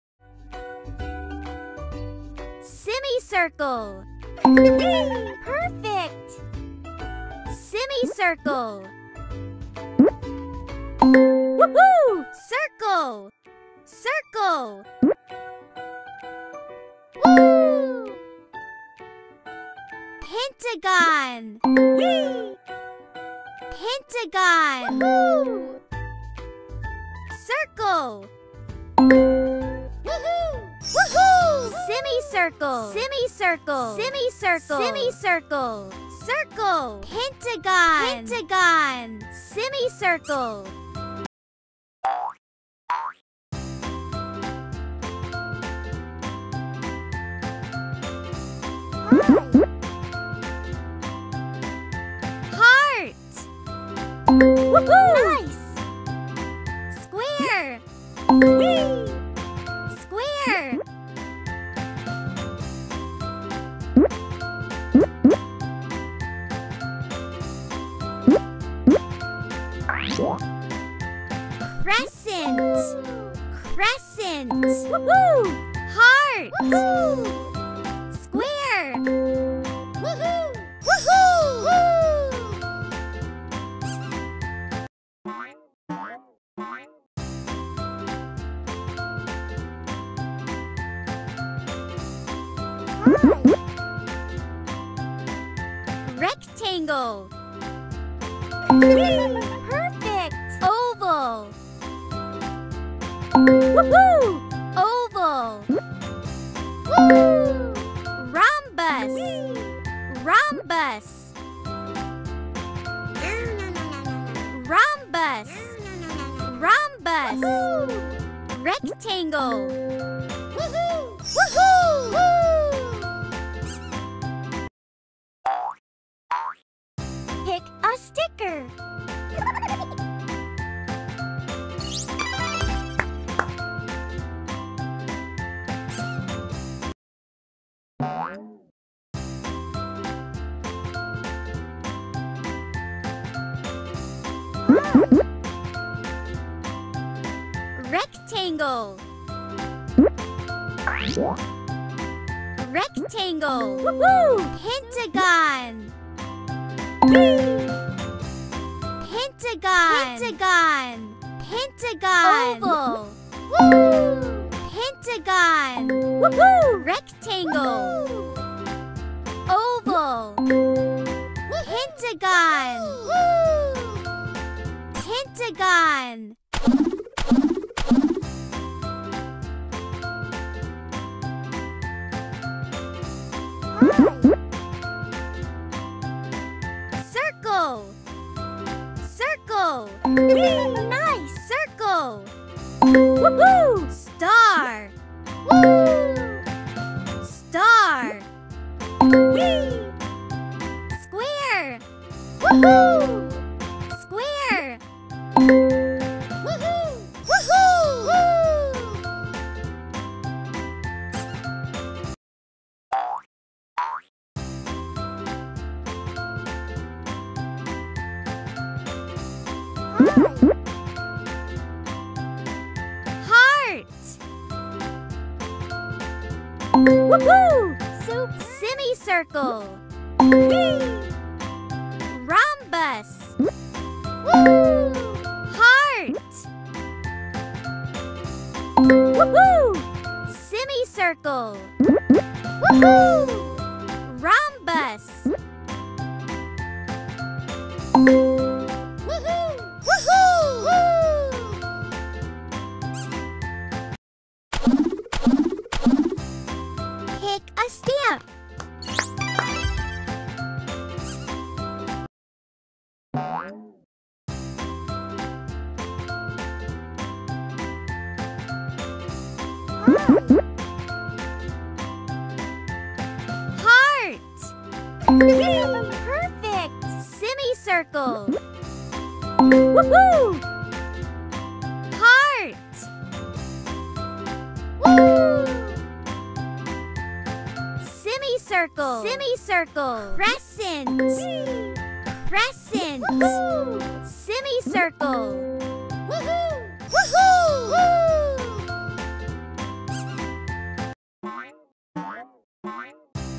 Sing Along! Shape & Color Party! (Learn Colors & Shapes for Kids)
Hey everyone, it's @allrhymeexplorers ! Today we're throwing a SHAPE & COLOR PARTY ! Join us for a wiggly, sing-along adventure where we learn all about circles, squares, triangles, and more! We'll sing catchy songs, see awesome animations, and even play some color games! 🟡

This video is perfect for toddlers and preschoolers to learn their colors and shapes in a fun and interactive way. Get ready to sing, dance, and become a shape and color master!